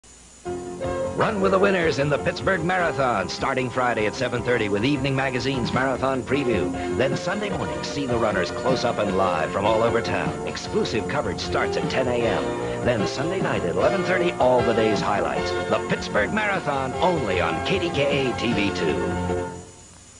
KDKA Pittsburgh Marathon coverage promo, 1986
Promo for Ch. 2's weekend coverage of the Steel City's annual running event.